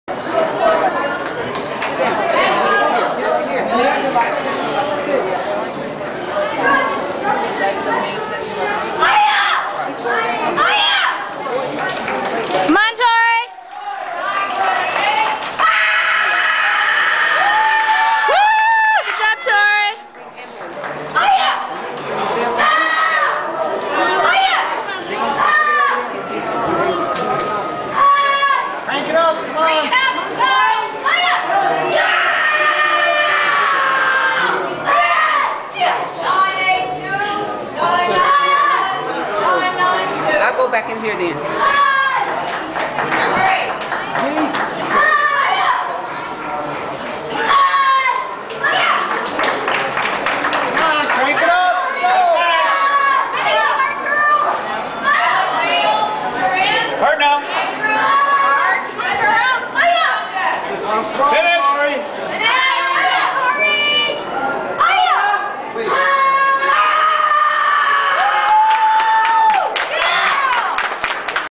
2008 Diamond Nationals - Tori - Brown Belt
- Tori a student at Nicklaus' Martial Arts America which provides karate lessons in La Crosse, WI and Onalaska, WI to help students develop leadership and character skills